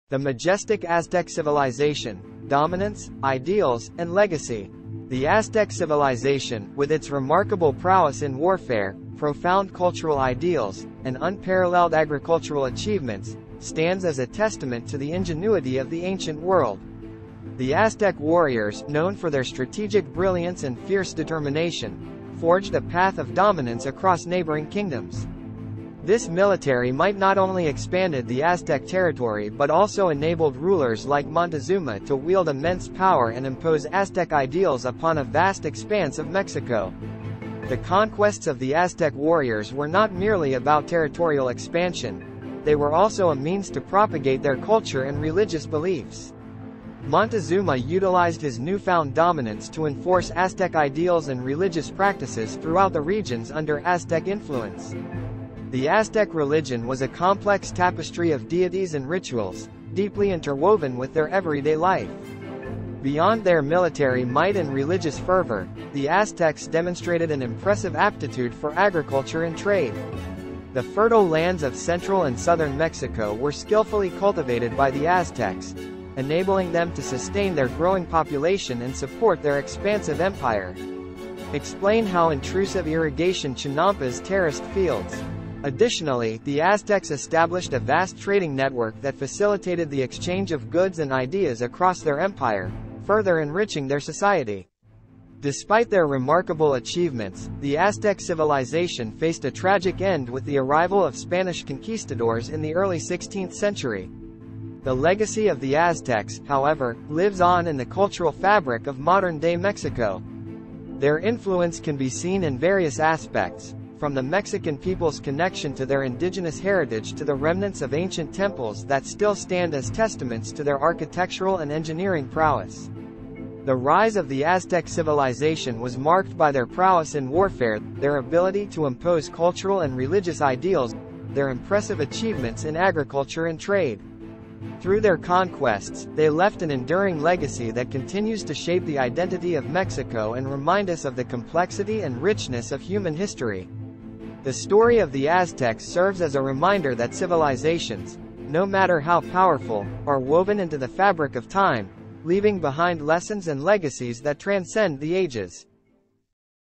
The Majestic Aztec Civilization Dominance, Ideals, and Legacy
Title: The Majestic Aztec Civilization: Dominance, Ideals, and Legacy

Introduction

The Aztec civilization, with its remarkable prowess in warfare, profound cultural ideals, and unparalleled agricultural achievements, stands as a testament to the ingenuity of the ancient world. Flourishing in the heartland of Central and southern Mexico from 1345 to 1521 AD, the Aztecs left an indelible mark on history through their military might, religious practices, and economic sophistication.

Warriors of Dominance

One of the defining characteristics of the Aztec civilization was its prowess in warfare. The Aztec warriors, known for their strategic brilliance and fierce determination, forged a path of dominance across neighboring kingdoms. This military might not only expanded the Aztec territory but also enabled rulers like Montezuma to wield immense power and impose Aztec ideals upon a vast expanse of Mexico. The Aztecs' military strategy involved a combination of disciplined troops, advanced weaponry, and tactical alliances, allowing them to conquer lands that stretched from modern-day Guatemala to the heart of Mexico.

Cultural Imposition and Religious Significance

The conquests of the Aztec warriors were not merely about territorial expansion; they were also a means to propagate their culture and religious beliefs. Montezuma, a prominent Aztec ruler, utilized his newfound dominance to enforce Aztec ideals and religious practices throughout the regions under Aztec influence. The Aztec religion was a complex tapestry of deities and rituals, deeply interwoven with their everyday life. Temples like the Templo Mayor were erected as grand symbols of their devotion, where rituals involving sacrifices played a central role in appeasing the gods and maintaining cosmic balance.

Agriculture and Trade: Pillars of Prosperity

Beyond their military might and religious fervor, the Aztecs demonstrated an impressive aptitude for agriculture and trade. The fertile lands of Central and southern Mexico were skillfully cultivated by the Aztecs, enabling them to sustain their growing population and support their expansive empire. Advanced irrigation systems, chinampas (floating gardens), and terraced fields showcased their mastery over agricultural techniques. Additionally, the Aztecs established a vast trading network that facilitated the exchange of goods and ideas across their empire, further enriching their society.

Legacy and Enduring Influence

Despite their remarkable achievements, the Aztec civilization faced a tragic end with the arrival of Spanish conquistadors in the early 16th century. The encounter between the Old World and the New World brought about a clash of civilizations, leading to the eventual downfall of the Aztec empire. The legacy of the Aztecs, however, lives on in the cultural fabric of modern-day Mexico. Their influence can be seen in various aspects, from the Mexican people's connection to their indigenous heritage to the remnants of ancient temples that still stand as testaments to their architectural and engineering prowess.

Conclusion

The Aztec civilization's rise to prominence was marked by their prowess in warfare, their ability to impose cultural and religious ideals, and their impressive achievements in agriculture and trade. Through their conquests, they left an enduring legacy that continues to shape the identity of Mexico and remind us of the complexity and richness of human history. The story of the Aztecs serves as a reminder that civilizations, no matter how powerful, are woven into the fabric of time, leaving behind lessons and legacies that transcend the ages.